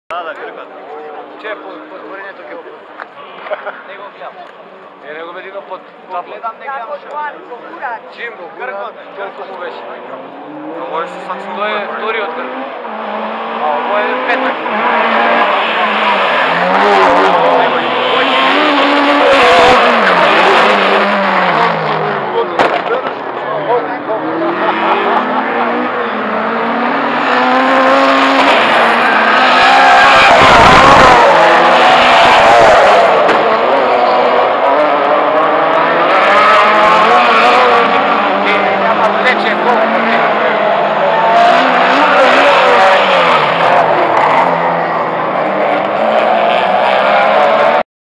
Race Day - Circuit Race - MRT 2010 - 18 of 59
Kruzna Trka kaj televizija vo Skopje na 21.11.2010
Kamera: Toshiba Camileo S10